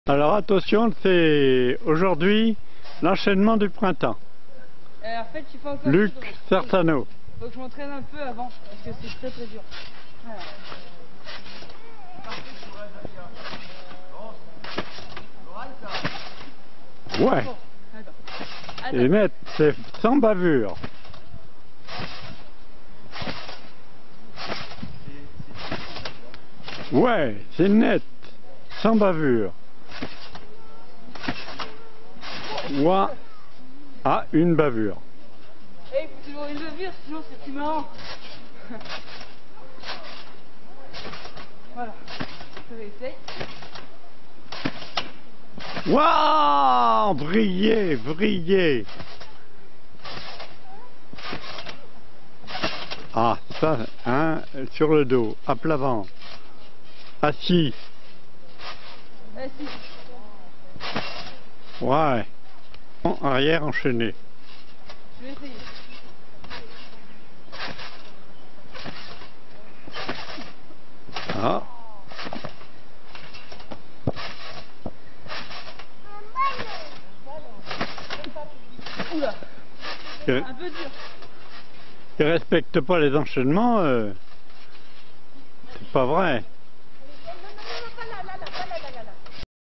Enchainement du printemps
Le 15 mars 2014 Luke travaille ses enchainements